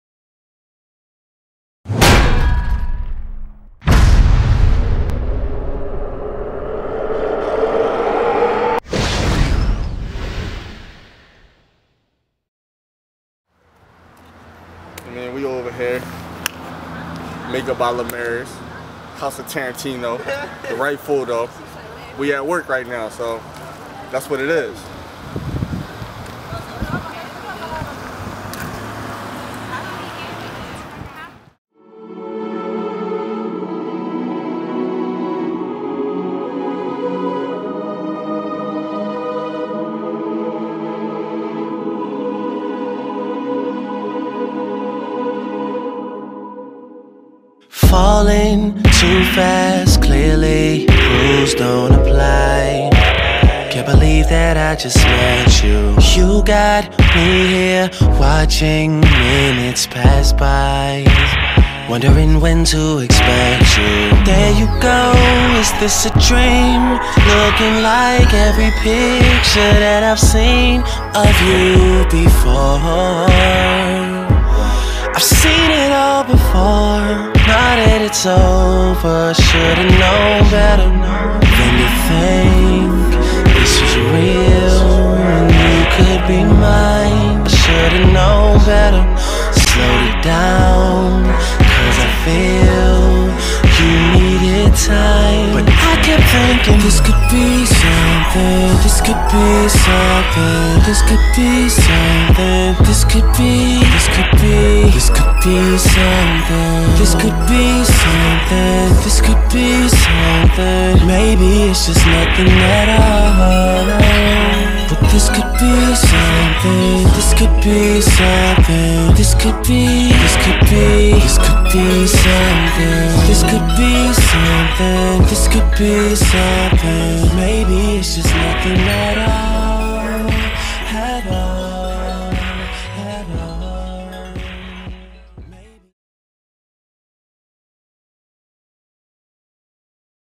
H.O.T PRESENTS: BEHIND THE SCENES WITH THE WRIGHT PHOTO
THE HOUSE OF TARANTINO TAKES YOU BEHIND OF THE SCENES OF THE WORLD OF PHOTOGRAPHY WITH

THE WRIGHT PHOTO - LARRY WRIGHT
AND
MAKE BY LIMARIS

PHOTOSHOOT: WEST SIDE STORY

DIRECTED & EDITED BY SAMMY TARANTINO